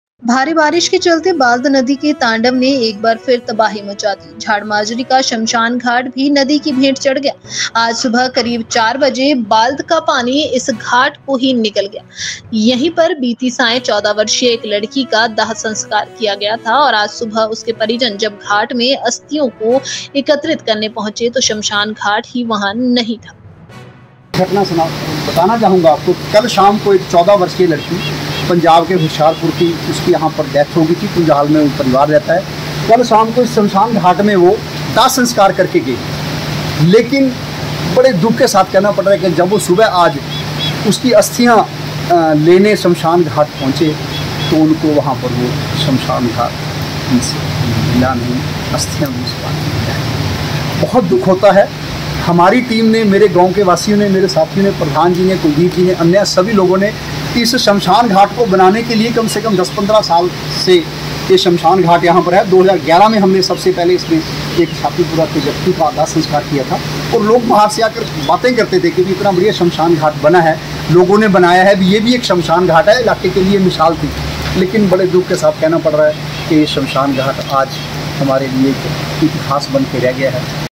Baddi | Cremation Ground | Jharmajri
Baddi (Himachal) : Due to heavy rains, the Tandav of Balad river once again created havoc. The cremation ground of Jhadmajri also succumbed to the river. This morning at about four o'clock the water of Bald swallowed this ghat itself.